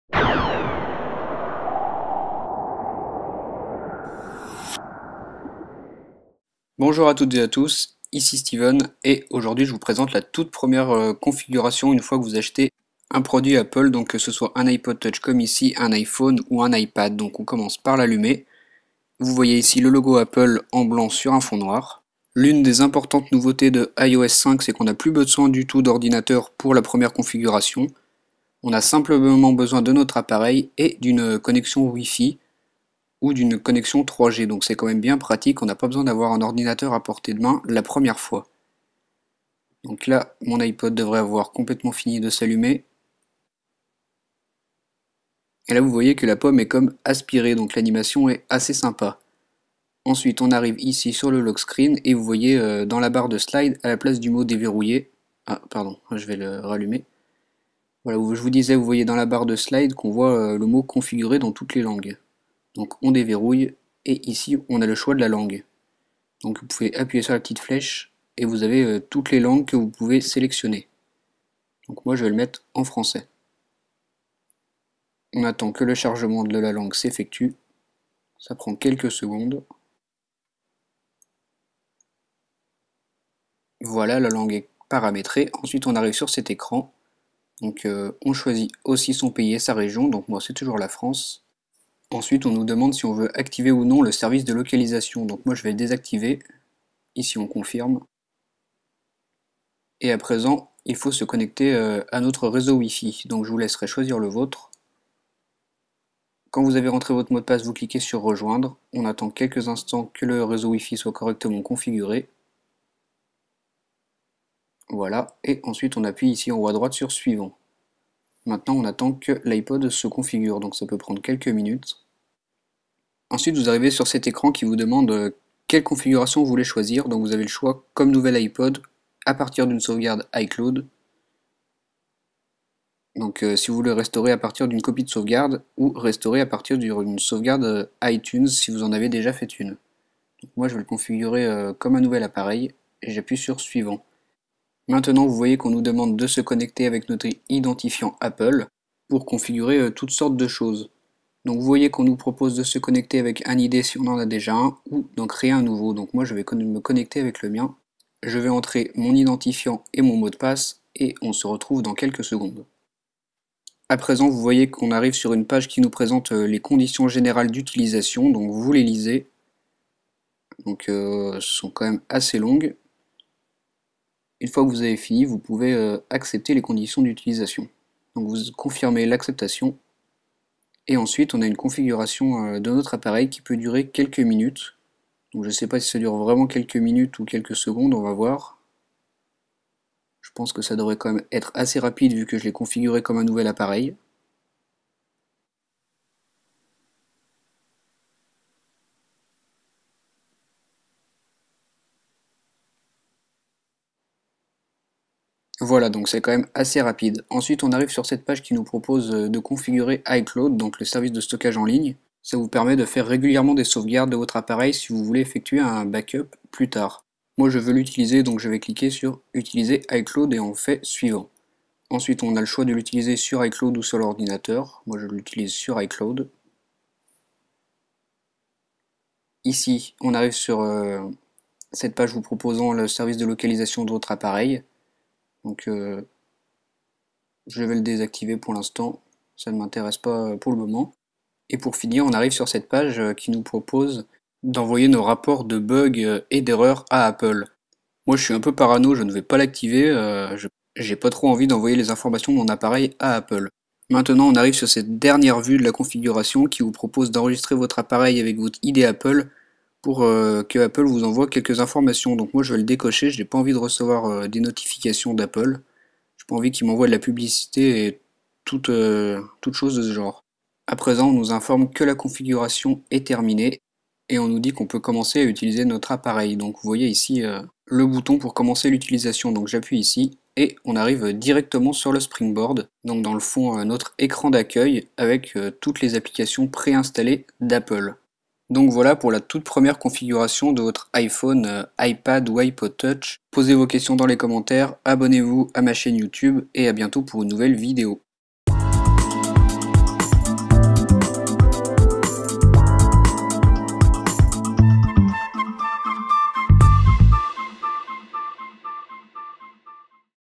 Première configuration d'un iPhone ou iPod Touch ou iPad
Je vous explique comment configurer votre tout nouvel iPhone, iPod Touch ou encore iPad, fraichement acheté et déballé.